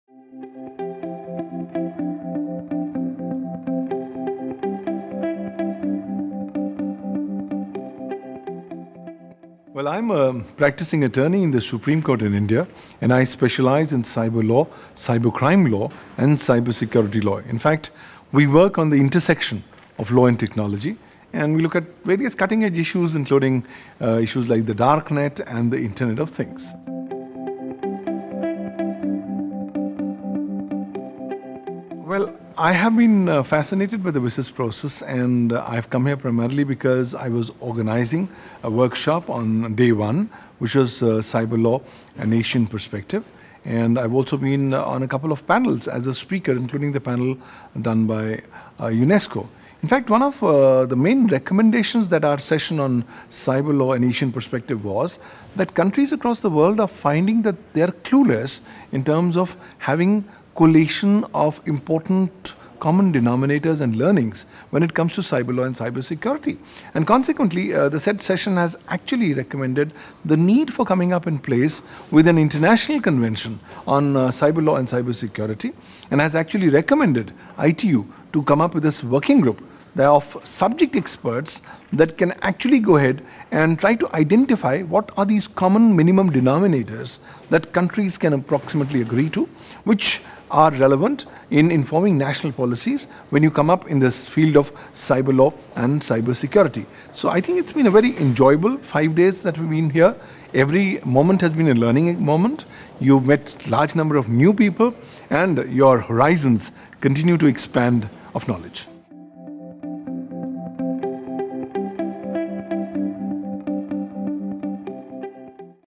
WSIS FORUM 2015 INTERVIEWS: Pavan Duggal, Attourney, Supreme Court of India
Interview with  Pavan Duggal, Attourney, Supreme Court of India WSIS FORUM 2015, Geneva, Switzerland. 25-29 May 2015.